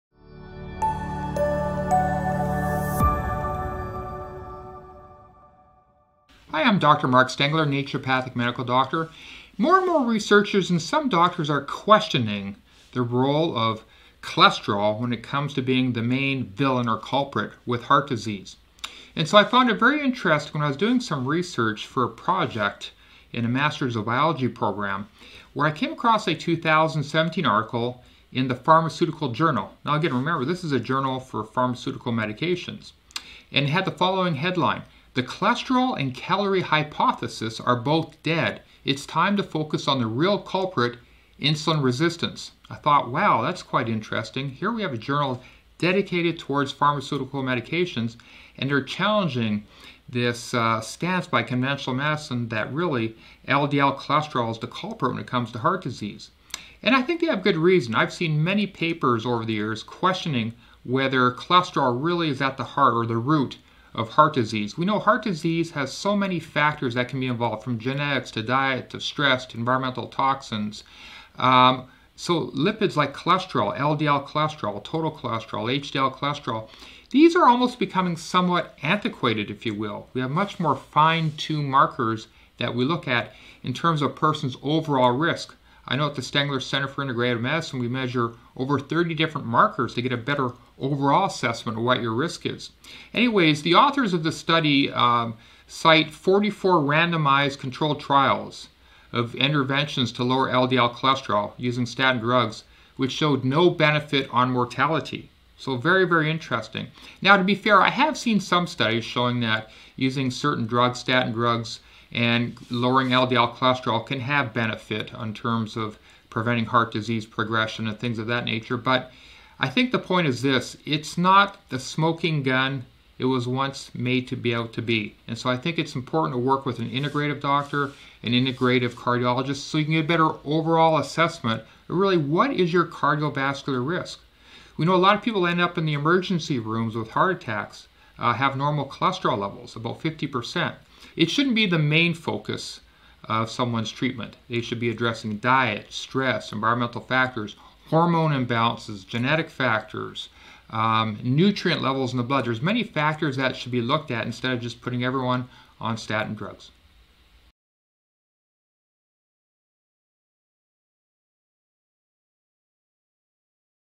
Is Cholesterol Actually the Villain of the Heart?
Cholesterol is an essential component of every cell in your body and is needed to manufacture hormones and other life-giving substances. Although cholesterol is obtained through high-fat and/or high-cholesterol foods, approximately 85% of cholesterol in the body is manufactured by the liver and, to a lesser degree, the small intestine.

Watch this video to better understand Cholesterol and how it affects our Hearts!!